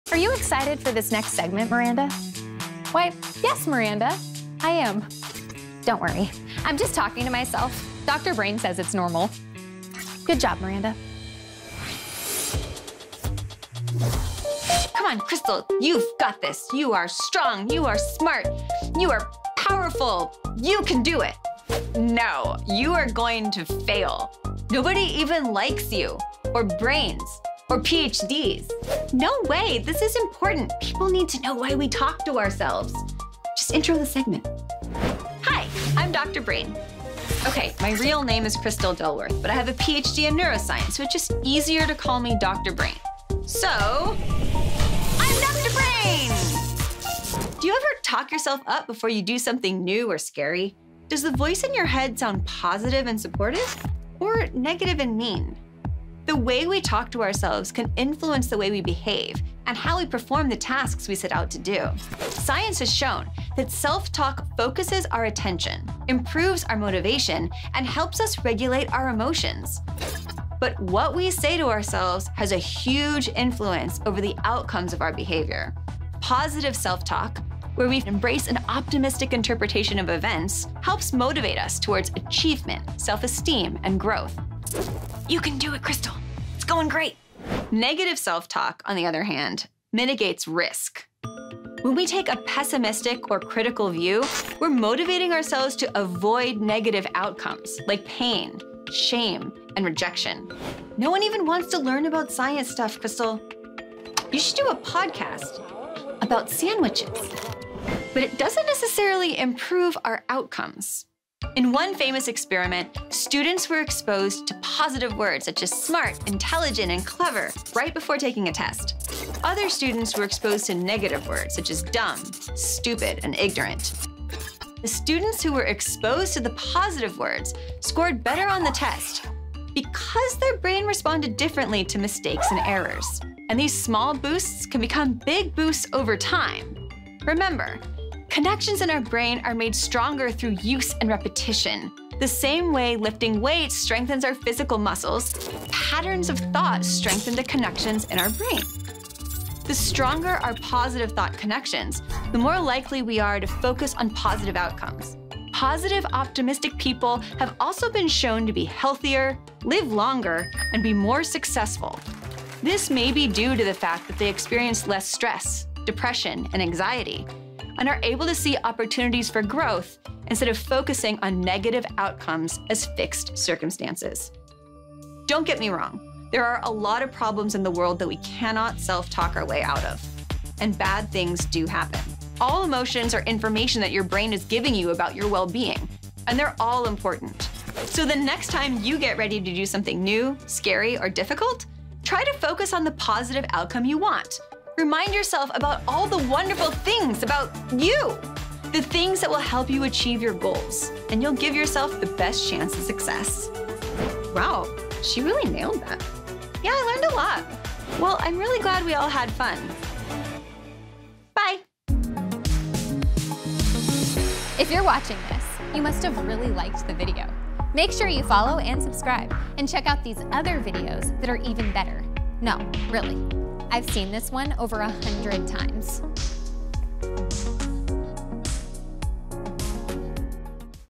Talking To Yourself Is Good For Your Brain
This is your brain on positivity! ☺️🧠✨ Neuroscientist Dr. Brain aka Crystal Dilworth explains how positive psychology can make a real impact on our brain's well-being.